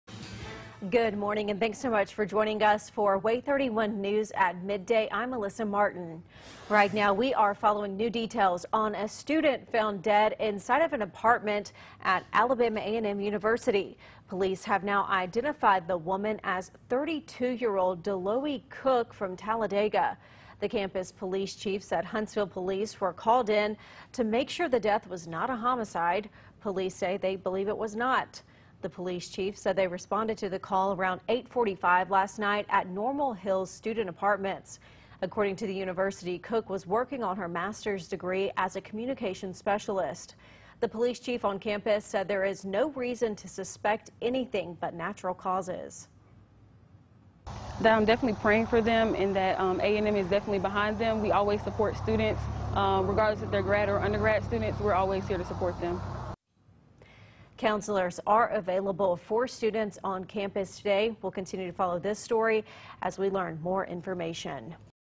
AAMU student found dead
Female Grad student found dead in student housing.